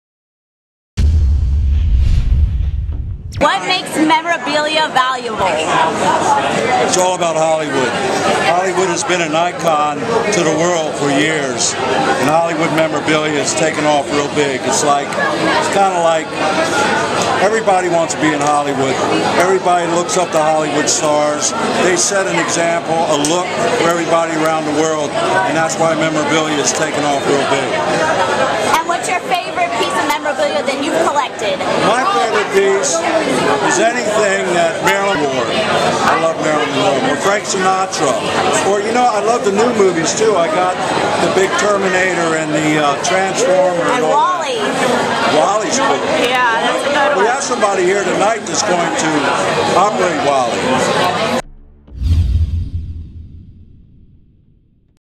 Treasure King Premiere
Richie Marcello of Treasure King talks to RumorFix.com about his favorite Hollywood Memorabilia.